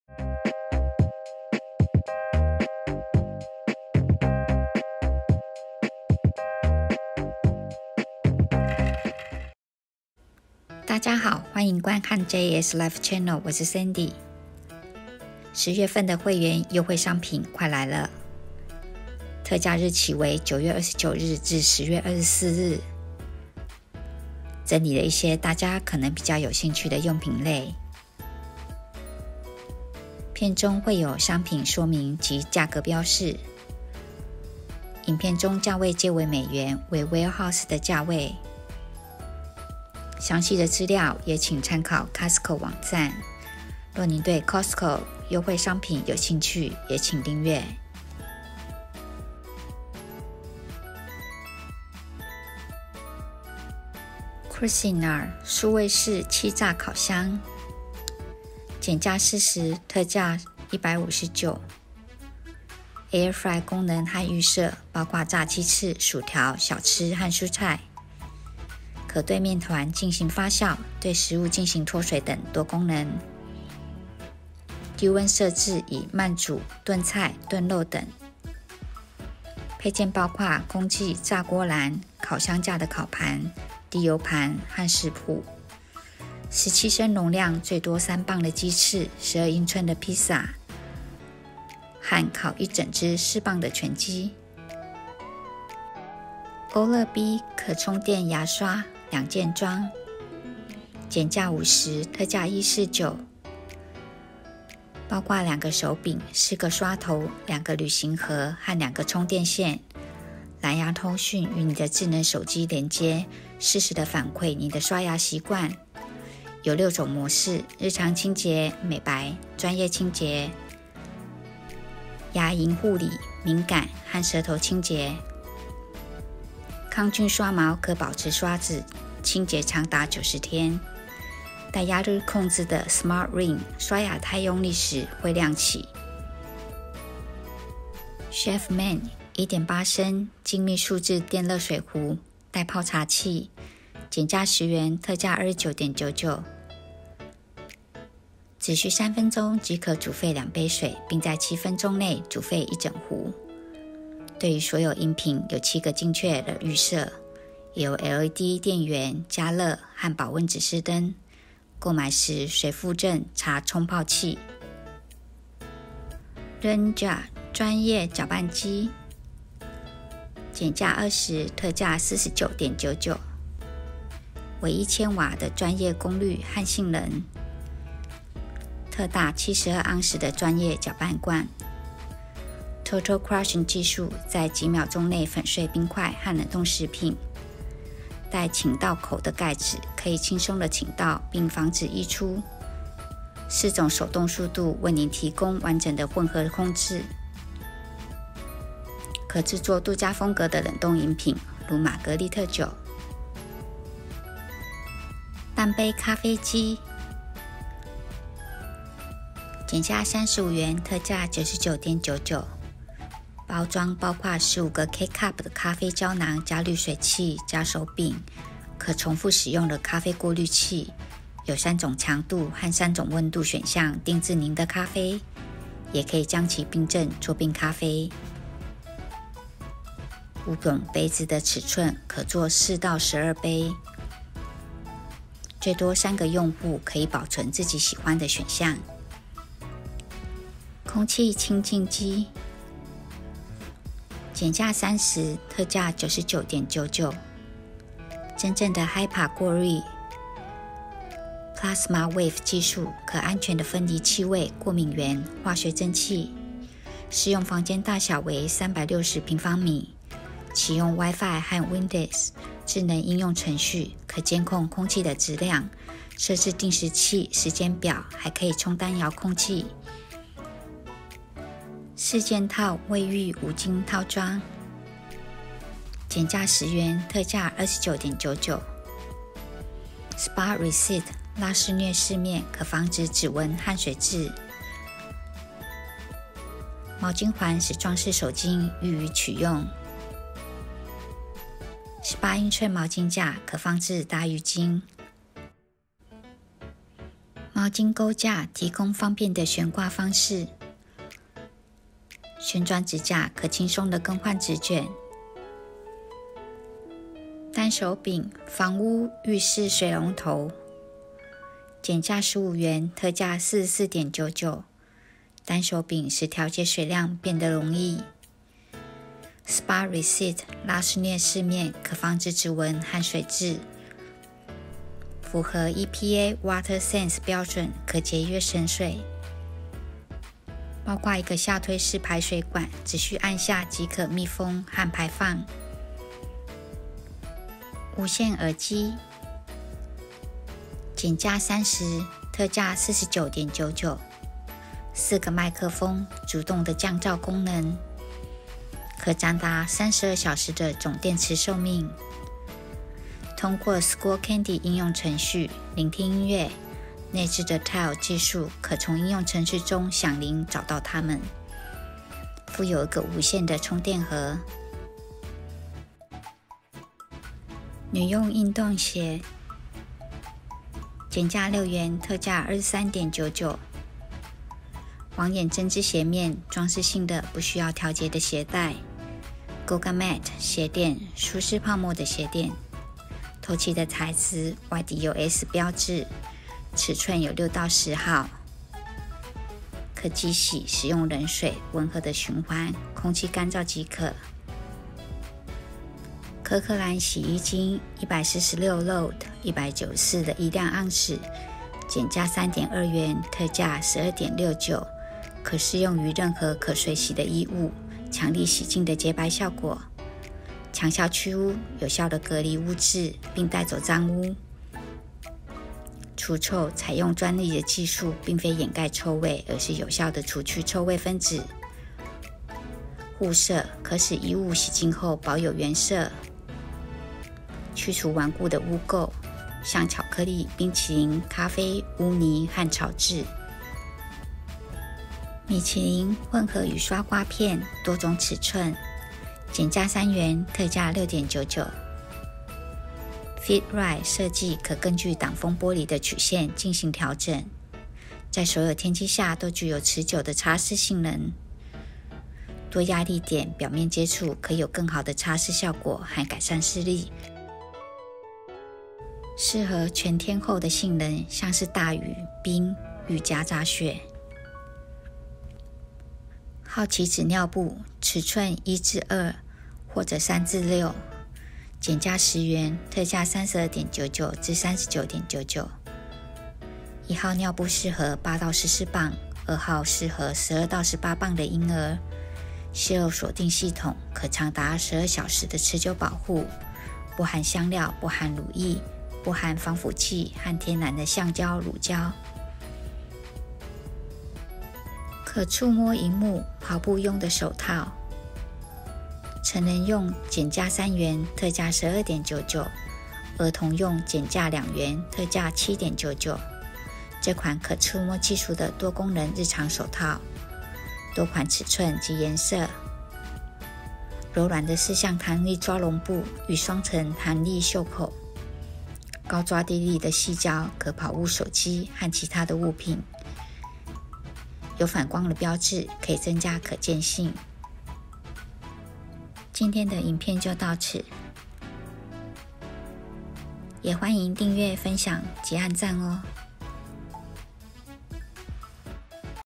Costco 2021/10月Member Savings| 1年才有1次減價的 Cuisinart（美膳雅）數位式氣炸烤箱😍2021獨家新款｜Ninja專業攪拌機｜Winix 空氣清淨機…等用品類
美國 Costco member savings 2021/10月會員特價
這次整理一下用品商品
提供大家参考。
若您有任何建議或批評 ，也請留言。

9/29/ 2021- 10/24/2021
計價單位： 美元

🙏有任何建議或感興趣的商品類也請留言！